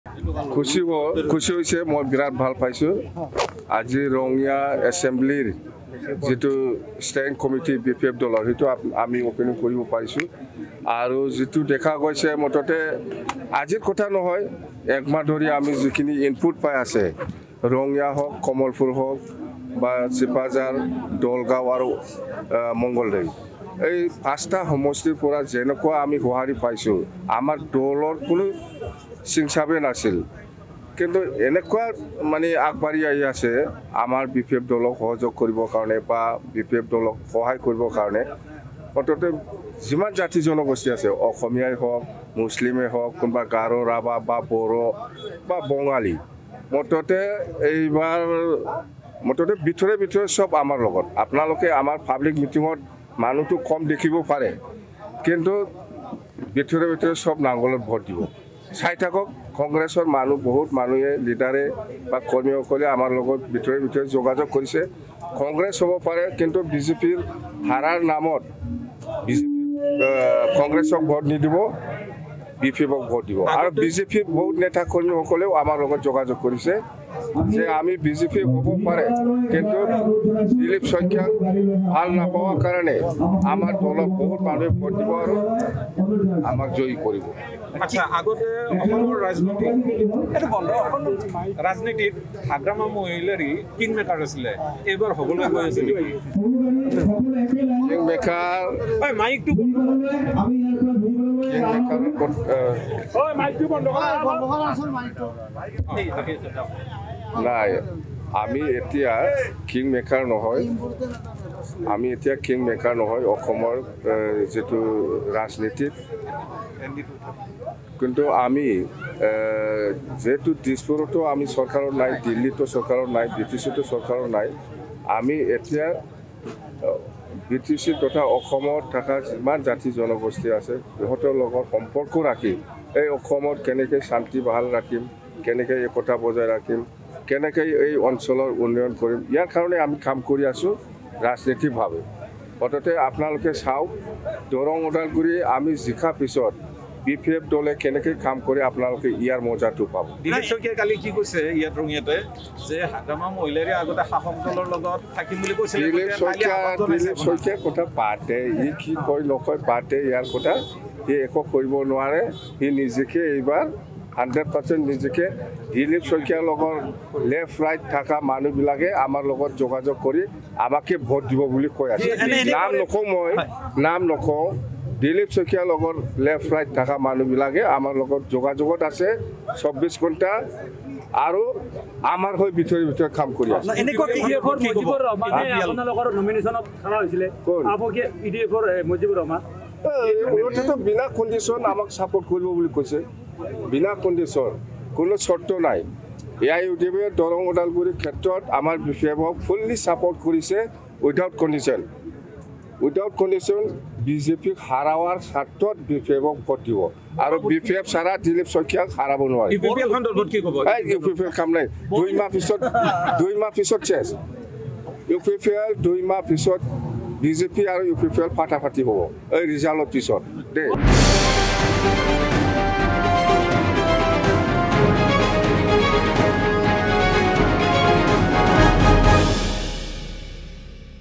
📌ৰঙিয়াত BPFৰ মুখ্য নিৰ্বাচনী কাৰ্যালয় উদ্বোধন।
📌দৰং ওদালগুৰি লোকসভা সমষ্টিৰ BPFৰ প্ৰাৰ্থী দূৰ্গা দাস বড়োৰ হৈ নিৰ্বাচনী প্ৰচাৰ চলালে হাগ্ৰামা মহিলাৰীয়ে।